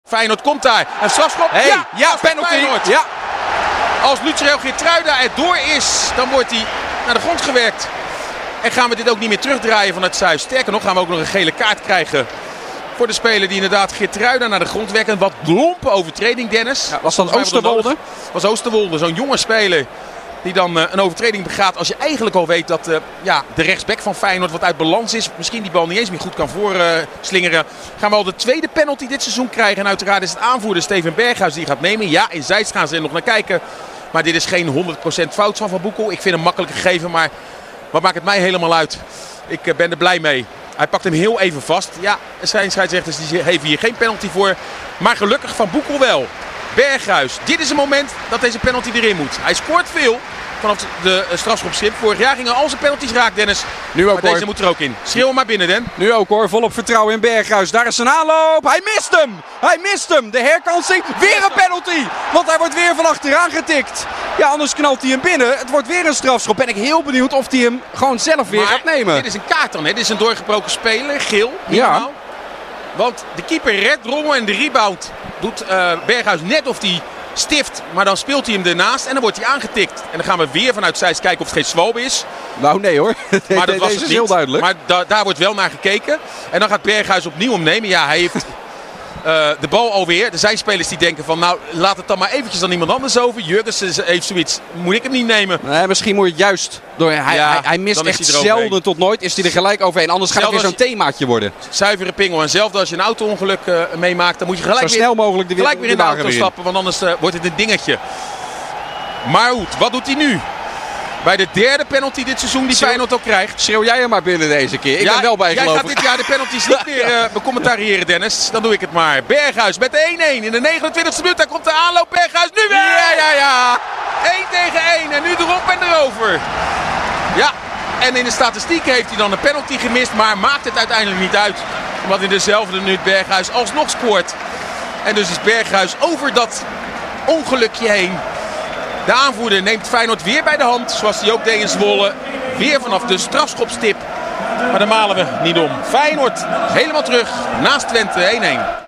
Zo klonk de 1-1 van Steven Berghuis bij Feyenoord-FC Twente op Radio Rijnmond
Wat een krankzinnige fase bij Feyenoord-FC Twente! De Rotterdammers kwamen op 1-1 via een strafschop, die benut werd door Steven Berghuis. Maar daar had hij wel een herkansing voor nodig. Zo klonk die gelijkmaker op Radio Rijnmond met het commentaar van Sinclair Bischop en Dennis van Eersel!